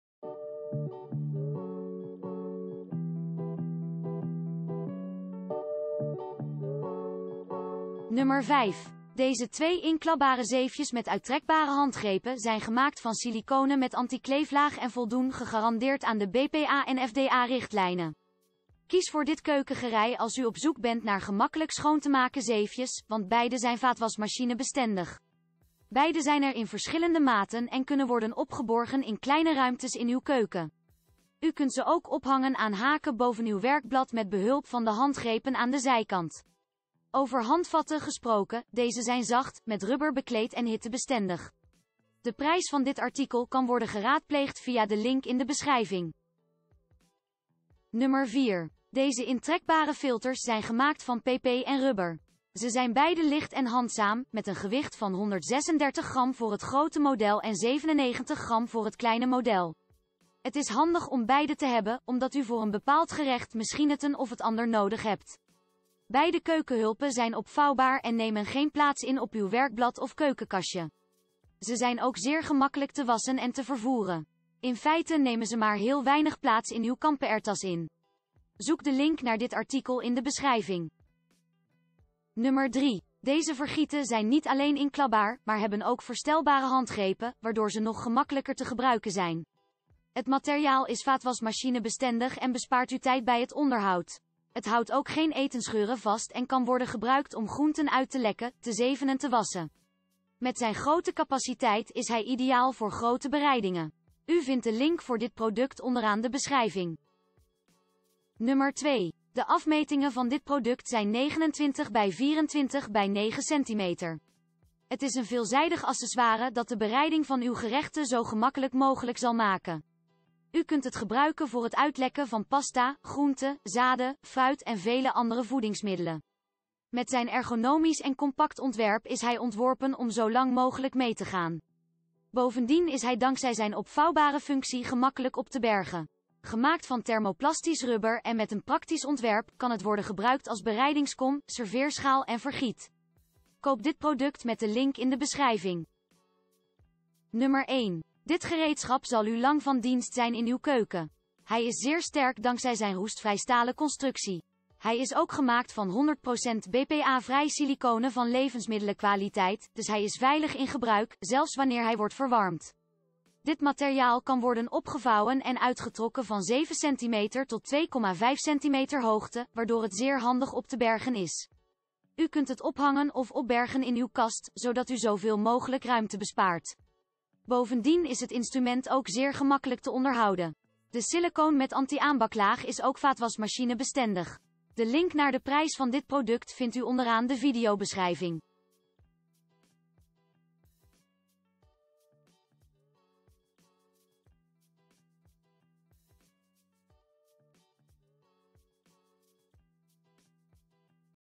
De 5 beste opvouwbare vergieten voor het uitlekken van fruit en groenten
1) INeibo Pastazeef opvouwba... »
2) Bilieasy - Set van 2 opvo... »
3) Itian Opvouwbare silicone... »
4) Diealles opvouwbare afdru... »
5) Ruesious Opvouwbaar vergi... »

✋ Hier de 5 beste opvouwbare vergieten voor het uitlekken van fruit en groenten, op de markt verkrijgbaar.

-- Tijdindicatoren (Tijdstempels) --

00:00 - Intro
00:08 - 5) Ruesious Opvouwbaar vergiet, siliconen inklapbare zeef, past...
00:47 - 4) Diealles opvouwbare afdruipzeef, siliconen, opvouwbare pasta...
01:22 - 3) Itian Opvouwbare siliconen zeef, zeef met uittrekbare handgr...
01:48 - 2) Bilieasy - Set van 2 opvouwbare keukenverf/opschuiver verfse...
02:27 - 1) INeibo Pastazeef opvouwbare keukenzeef van voedselveilige en...